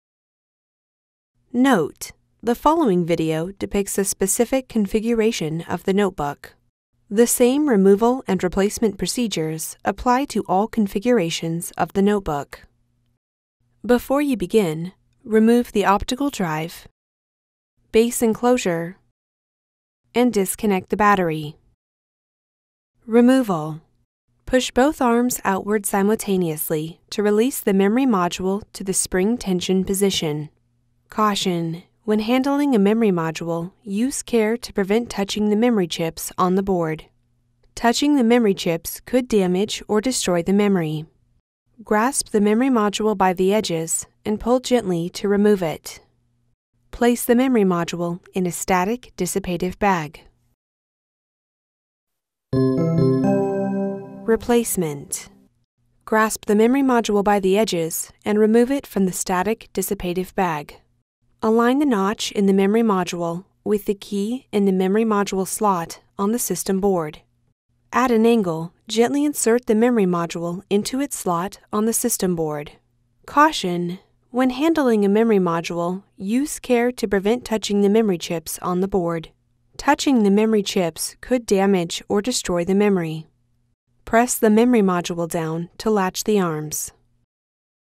Replace the Memory Module(S) | HP Pavilion 15-au000 notebooks | HP Support
Learn how to remove and replace the memory module(s) for HP Pavilion 15-au000 notebooks.